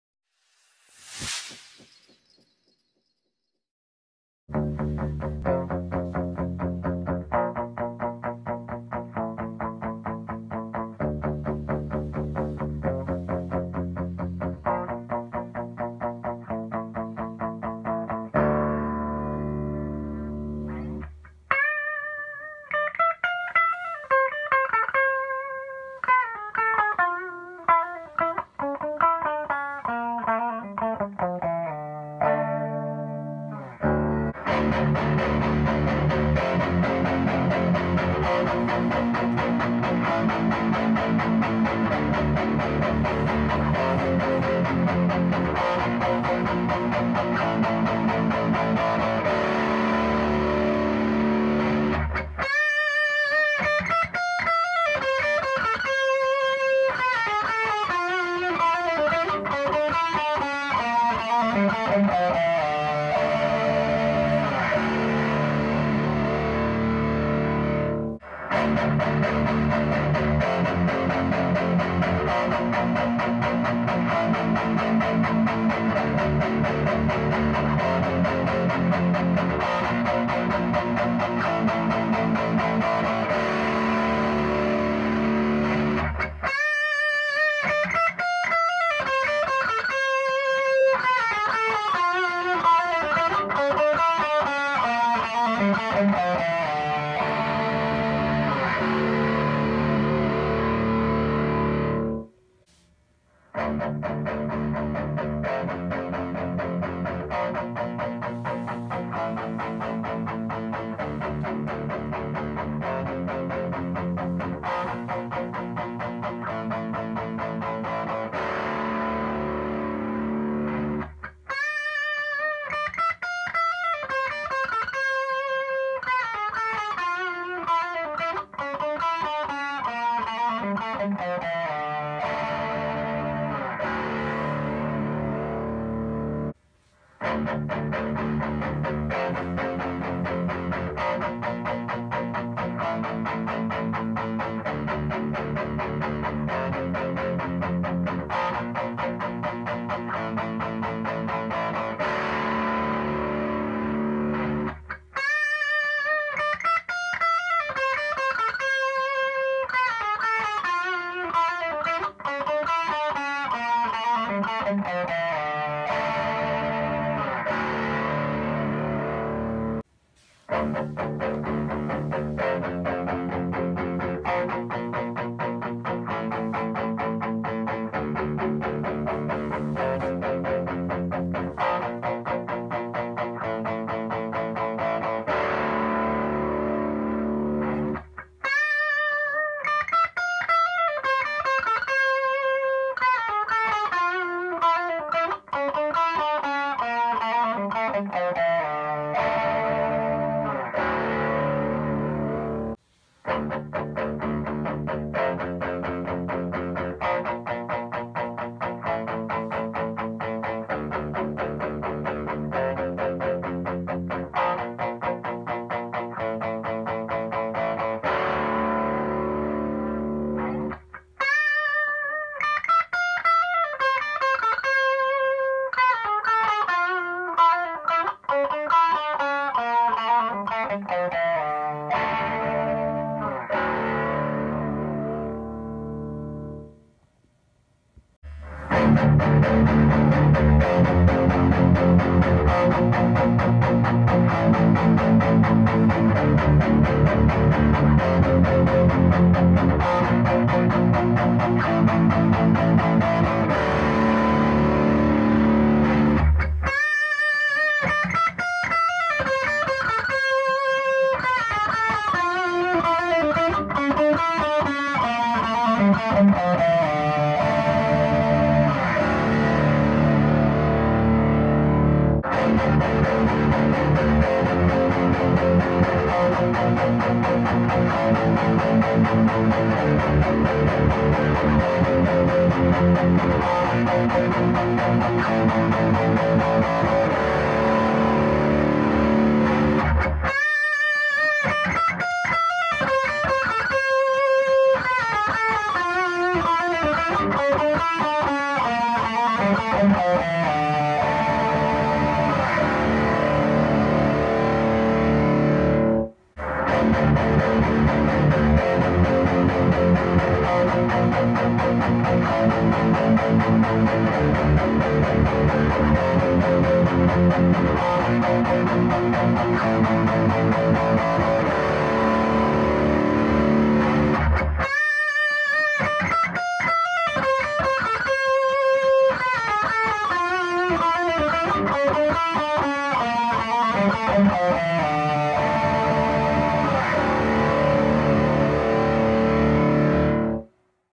Marshall The Guv'nor比較（England Korea〕
自分の持っているEngland製とKorea製の比較です。
聴き取りにくいかもしれませんが参考になれば。